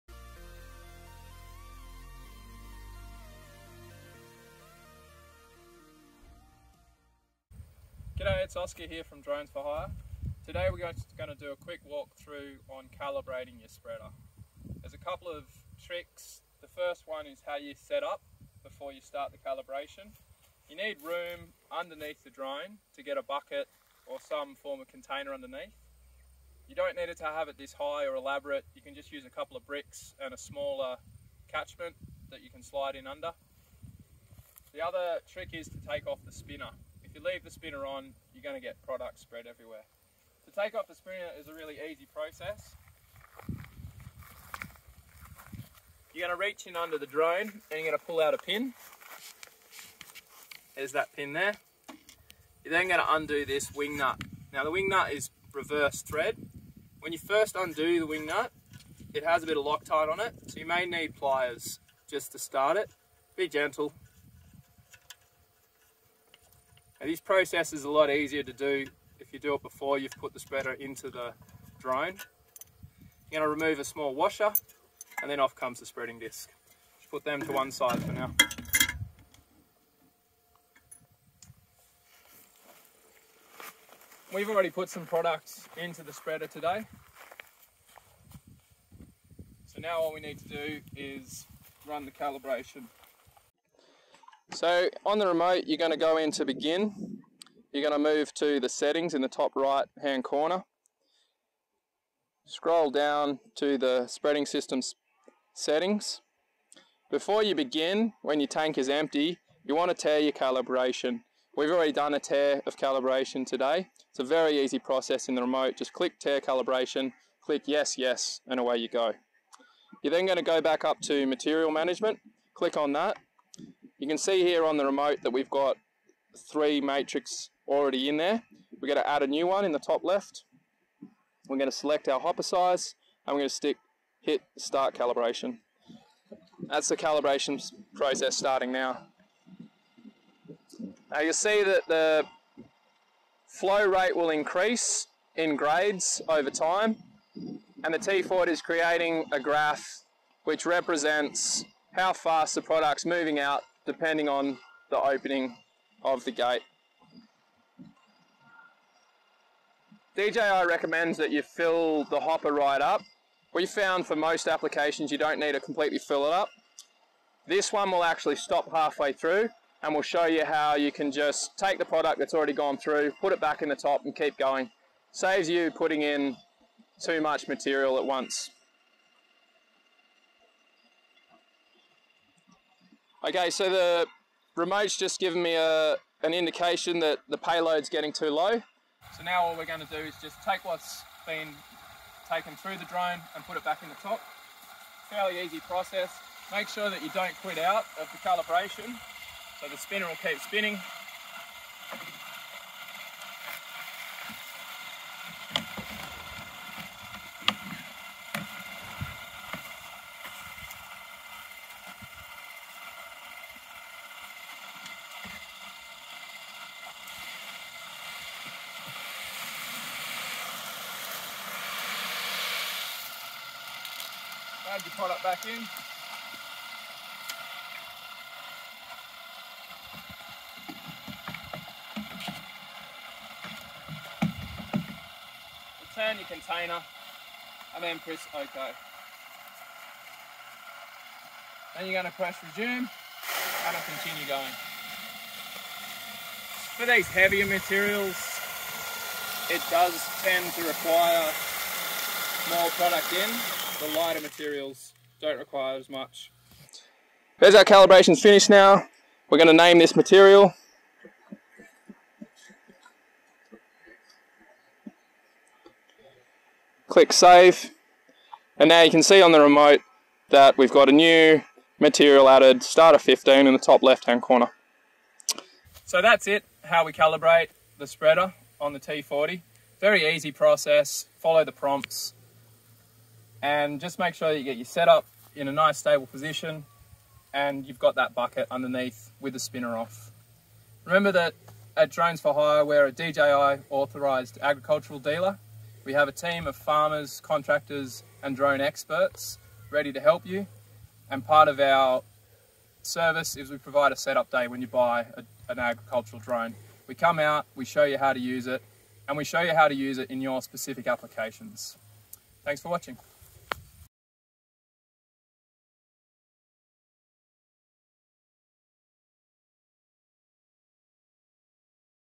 DJI agras T40 Spreader Calibration
A quick walk through on how to calibrate a DJI Agra T40 spreader by adding a new material to the material list.